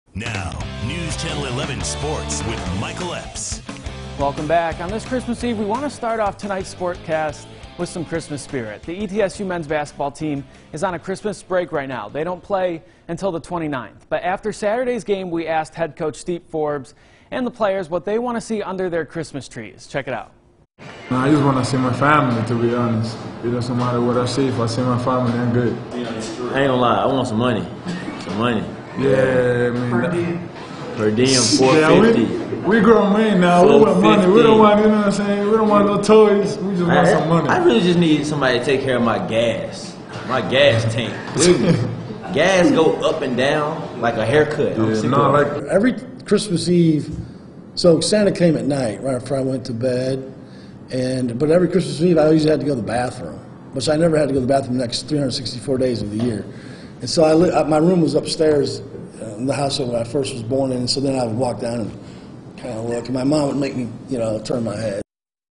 ETSU men's basketball describes what they want for Christmas
Jeromy Rodriguez, Tray Boyd III, Steve Forbes